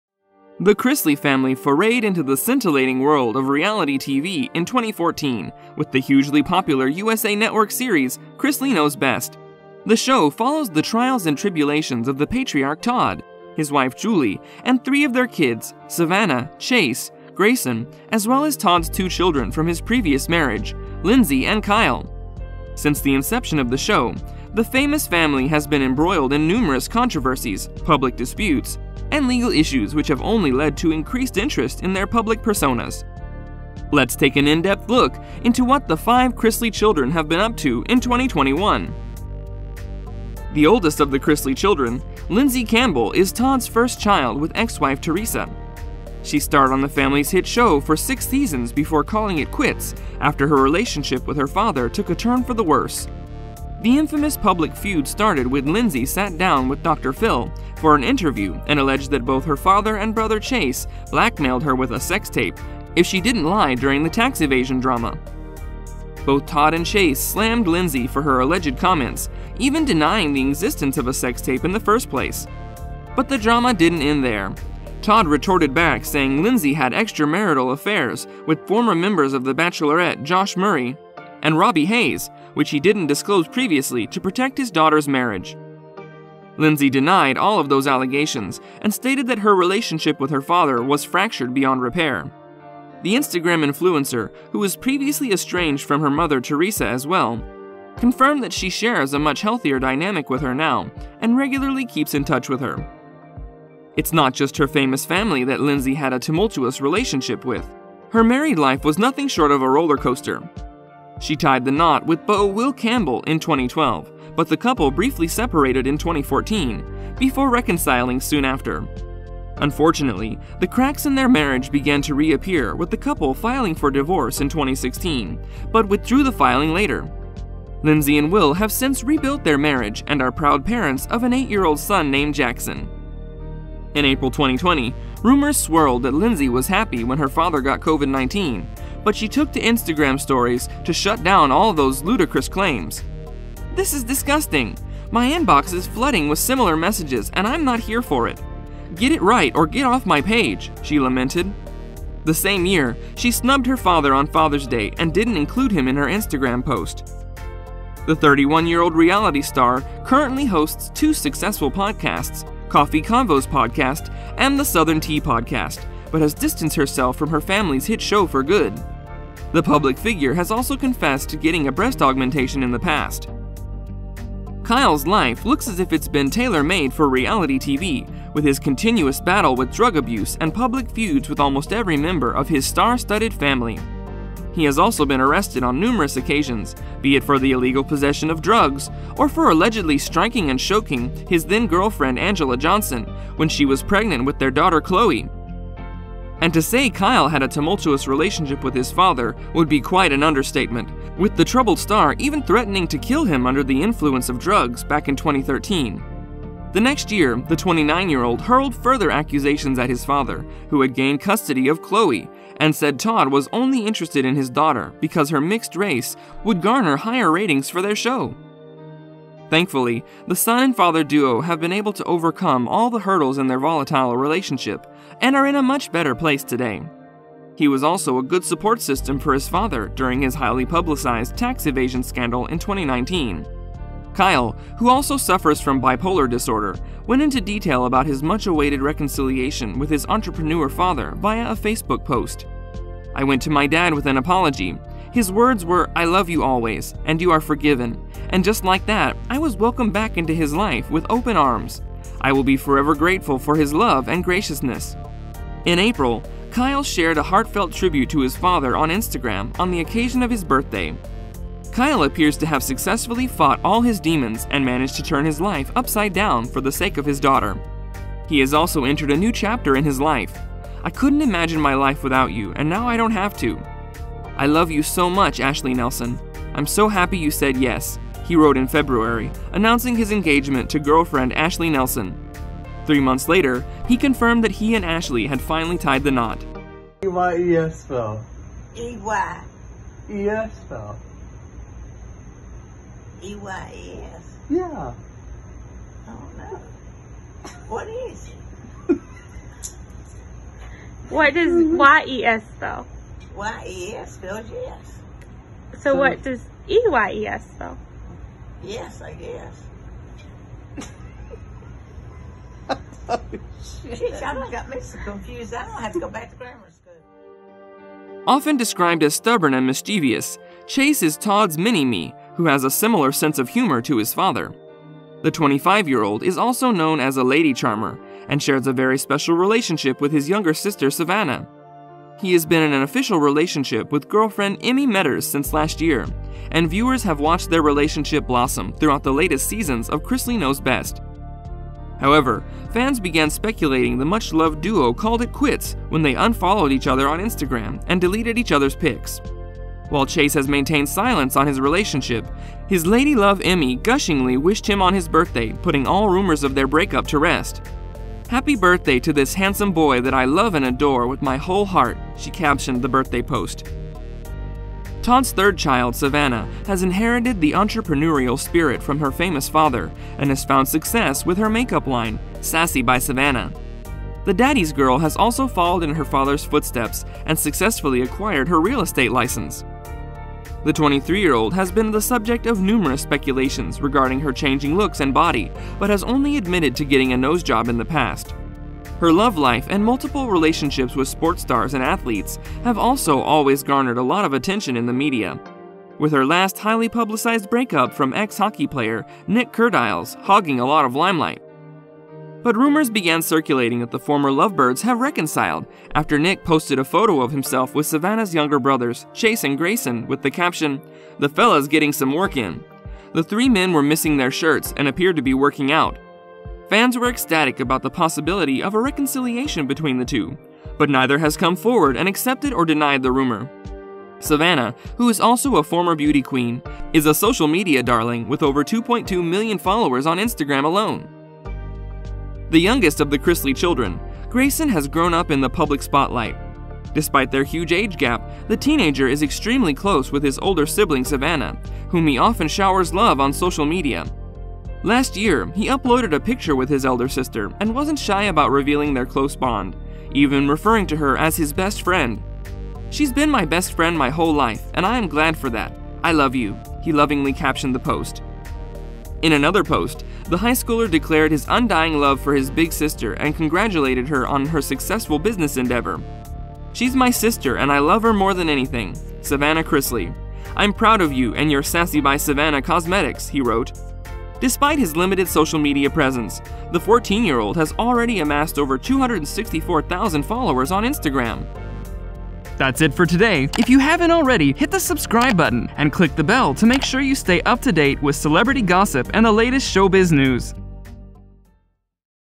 Todd Chrisley's All 5 Children 2021: New Relationship, Family Feud & Everything!
The Chrisley family forayed into the scintillating world of reality TV in 2014 with the hugely popular USA Network series Chrisley Knows Best. The show follows the trials and tribulations of the patriarch Todd Chrisley, his wife Julie, and 3 of their kids: Savannah, Chase, Grayson, as well as Todd’s 2 children from his previous marriage: Lindsie and Kyle. Since the inception of the show, the famous family has been embroiled in numerous controversies, public disputes, and legal issues which have only led to increased interest in their public personas. Let’s take an in-depth look into what the 5 Chrisley children have been up to in 2021.

Background Pictures: pixabay.com (No attribution required/ Free for Commercial Use)
Sources: Instagram and Twitter